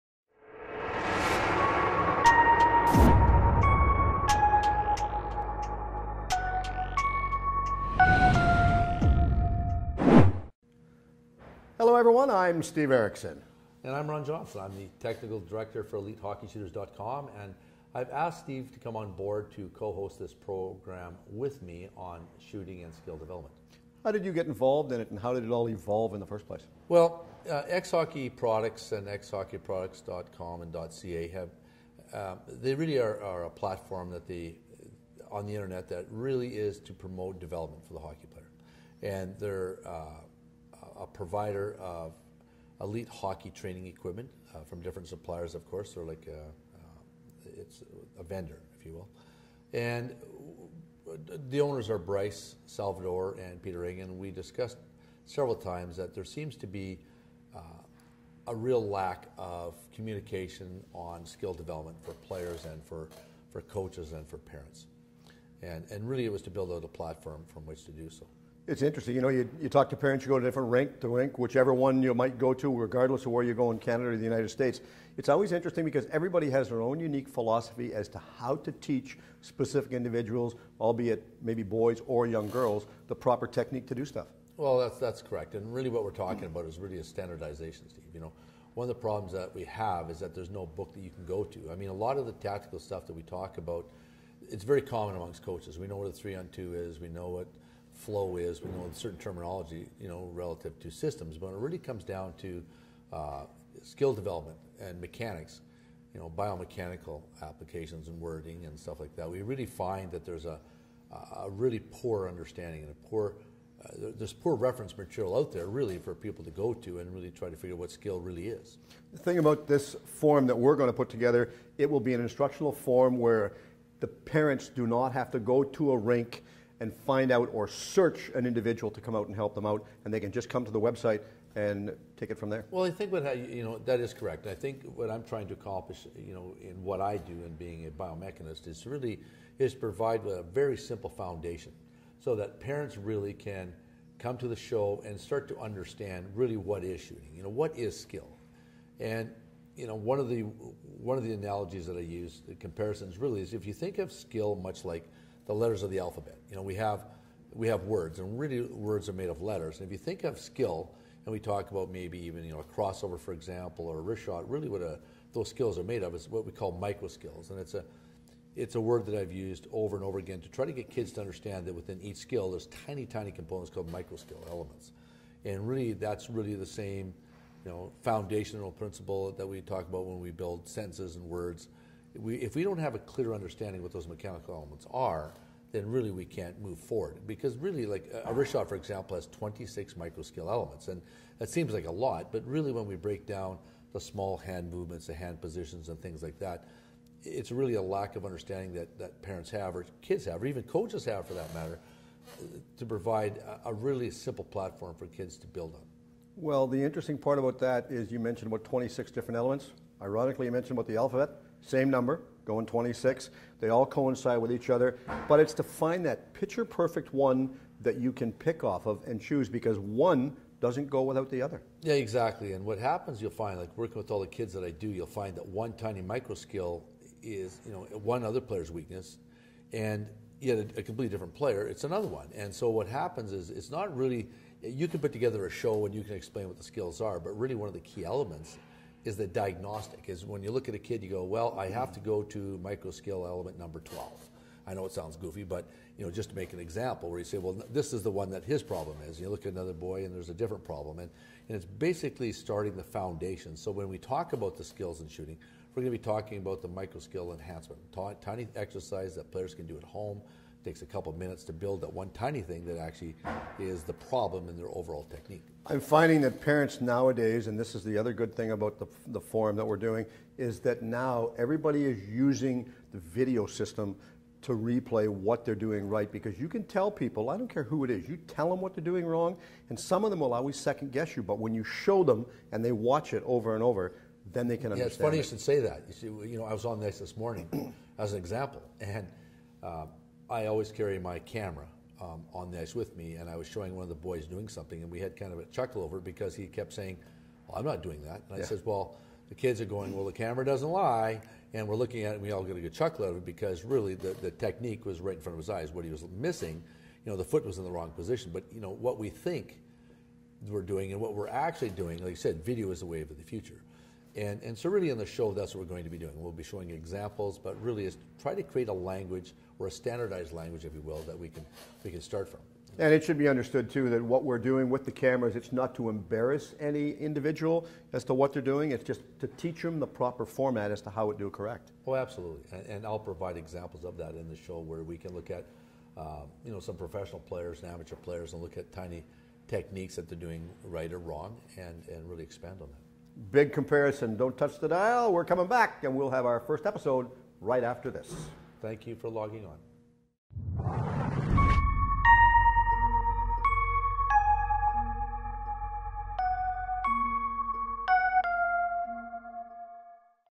XHockeyProducts Shot Doc Series with Ron Johnson - Introduction
Ron Johnson a.k.a "XHP Shot Doc" has partnered with XHockeyProducts to bring an educational shooting series to the XHockeyProducts Community. This video series is dedicated to the art of shooting and scoring goals!

The information in this series is based on Ron Johnson's EHS shooting philosophy, and how the standardization of terms and assessment and training of the micro-skills impact development.

Like all primary sports skills ranging from pitching and batting for baseball, to putting and driving for gold, shooting a hockey puck is a highly specialized skill. But it is often taken for granted! When it comes to the development of hockey players, shooting ranks quite low. The primary focus is the development of skating, with the secondary focus being on stickhandling skills.

XHockeyProducts, together with XHP Shot Doc - Ron Johnson, look to change this development approach and tackle this undeveloped hockey skill!